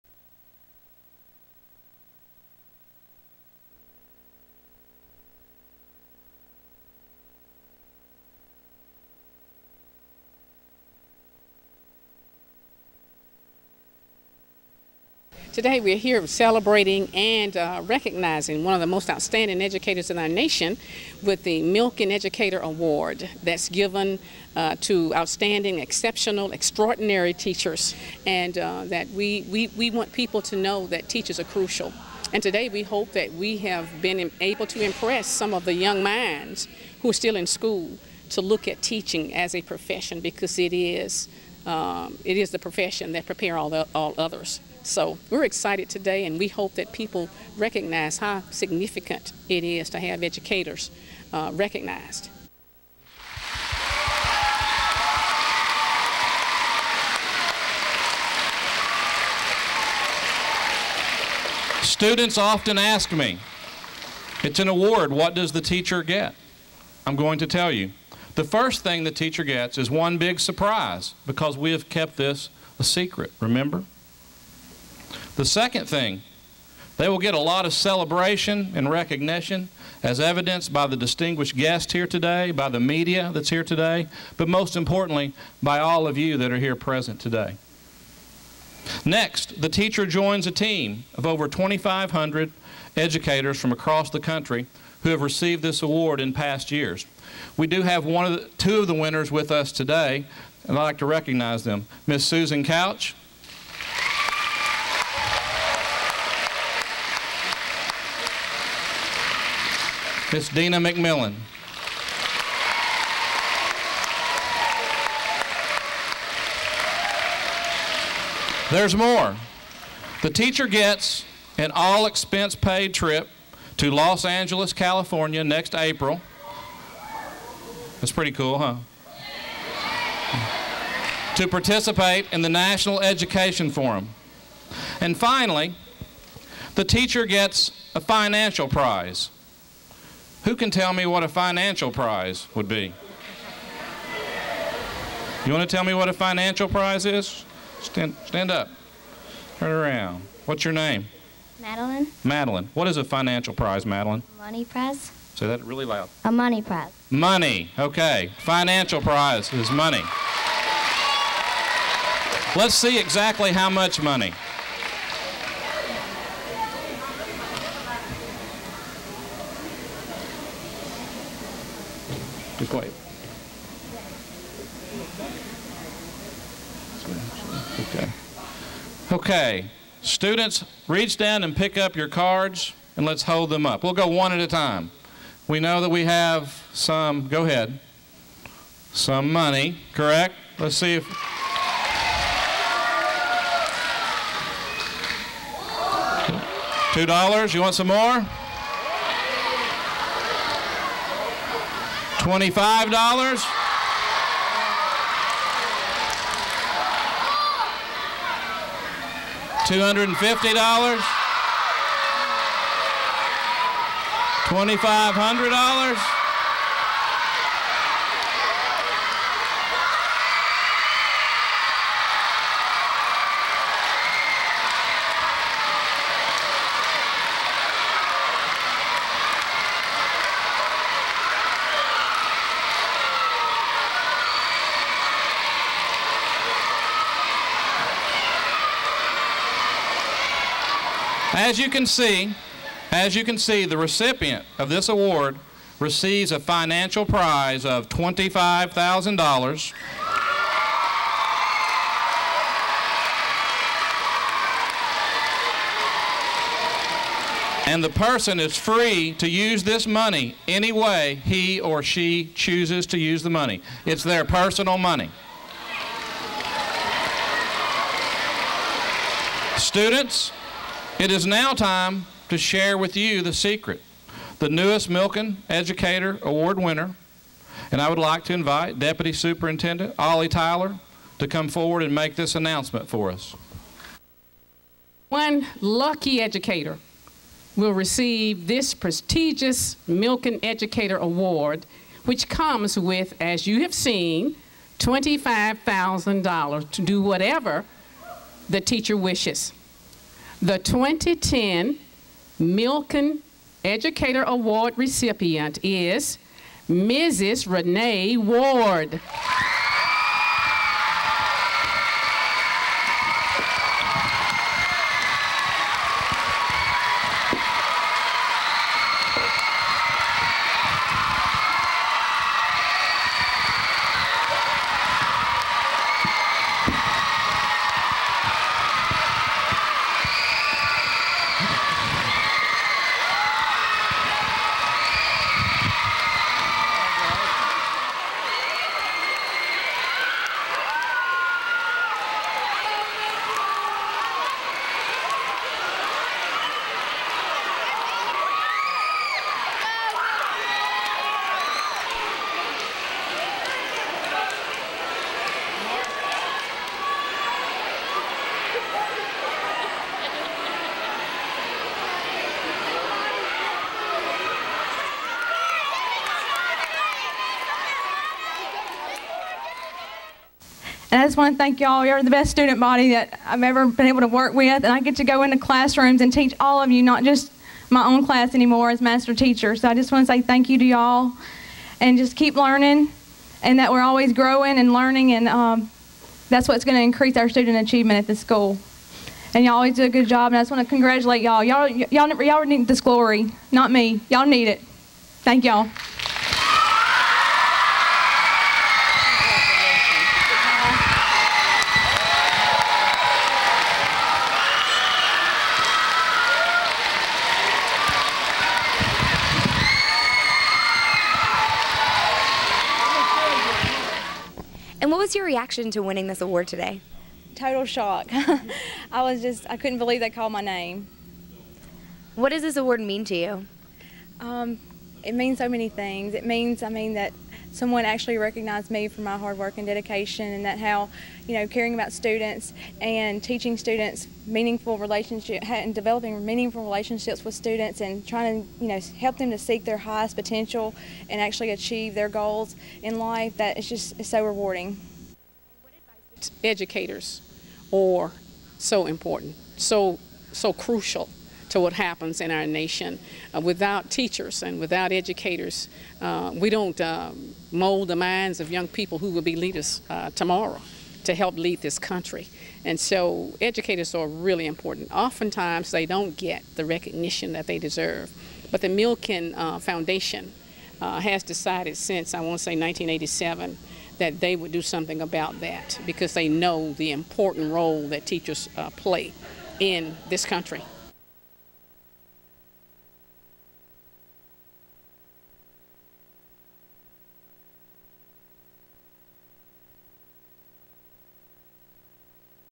2010 Milken Educator Award - Renee Ward
In a surprise ceremony at Delhi Charter School teacher Renee Lee Ward learned she was a recipient of the prestigious Milken Educator Award. The national award includes a $25,000 cash prize  to spend any way she wishes.
        Ward, a former first grade teacher with 11 years of experience, is in her first year as a Master Teacher at Delhi Charter School. In the classroom, she analyzes students' standardized tests and benchmark data to identify her students' areas of strength and weakness. In her role as Master Teacher, Ward utilizes research-based teaching strategies and works to provide a strong system of support to the teachers at her school.